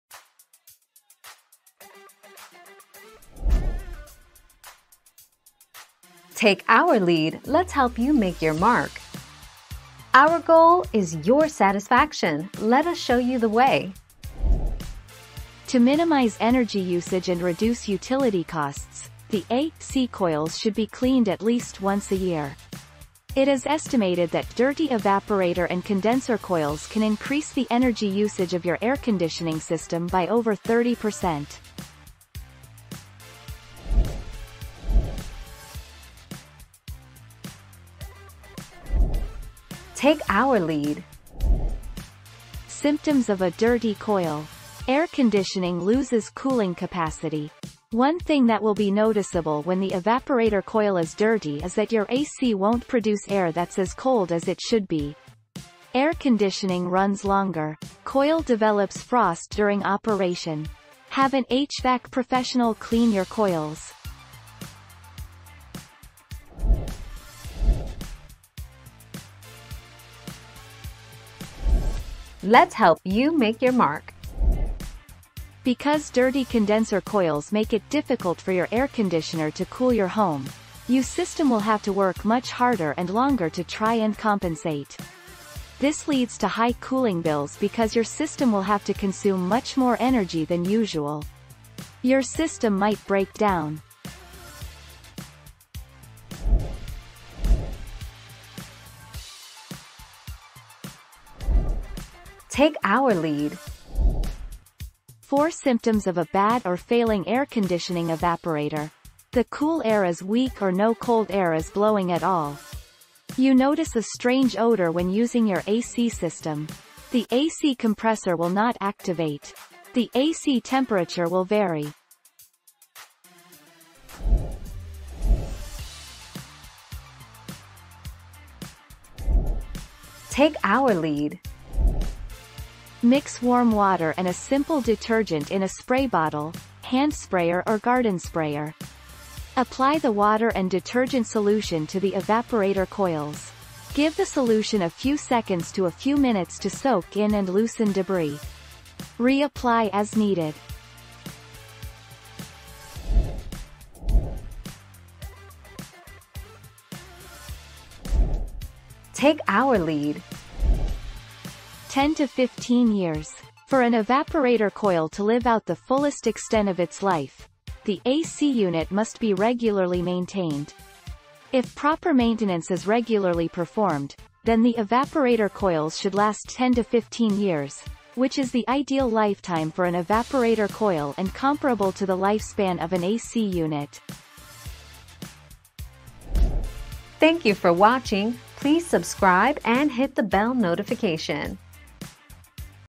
How often should AC evaporator coils be cleaned?
00:00 - How often should AC evaporator coils be cleaned?
00:35 - How do I know if my AC coil is dirty?
01:08 - What happens if AC coils are dirty?
01:38 - How do I know if my AC evaporator is bad?
02:05 - How do you clean an evaporator coil without removing it?
02:34 - How long does an AC evaporator coil last?

Laura S. Harris (2021, June 7.) How often should AC evaporator coils be cleaned?
    AskAbout.video/articles/How-often-should-AC-evaporator-coils-be-cleaned-251625